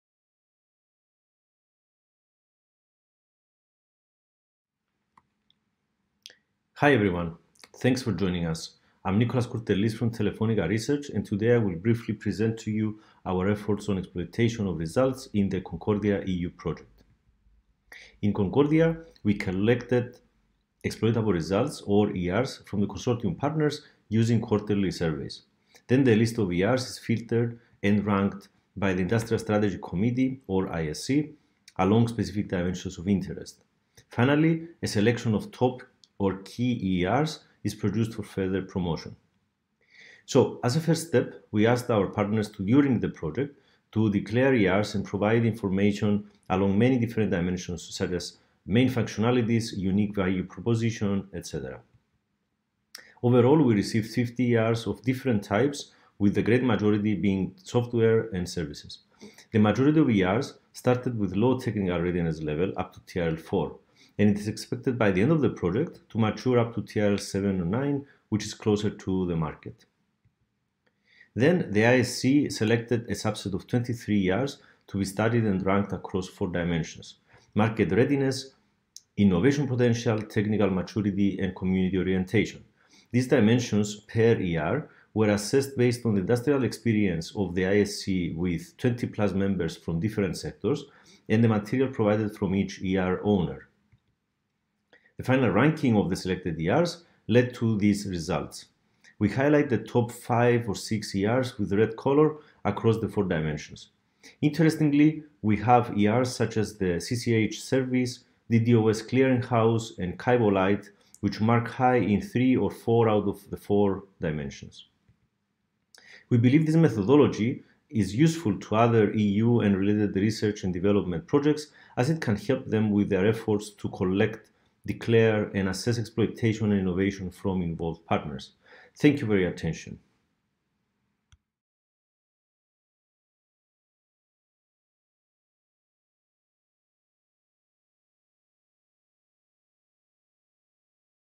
The Exploitation of Results - CONCORDIA Delivers
In this video, Nicolas Kourtellis from Task 5.1 talks about the exploitable results of the CONCORDIA project.